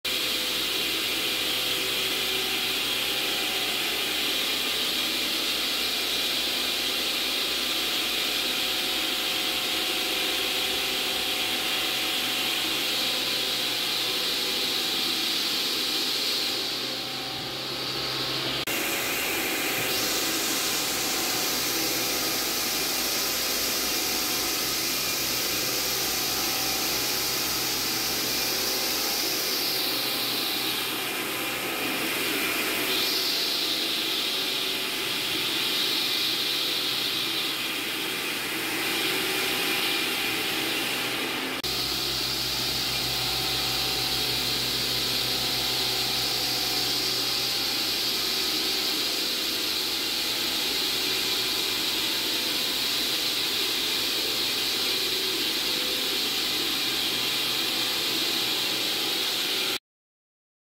Shot Blasting
Shot Blast to remove contaminants, paints, sealants, and laitance. It prepares floors for food processing, manufacturing, bridges, roadways, parkades, and waterproofing.

Di-Tech International
Innovators of Concrete Cutting and Removal
Reduce Labour - Increase Safety - Save Time & Money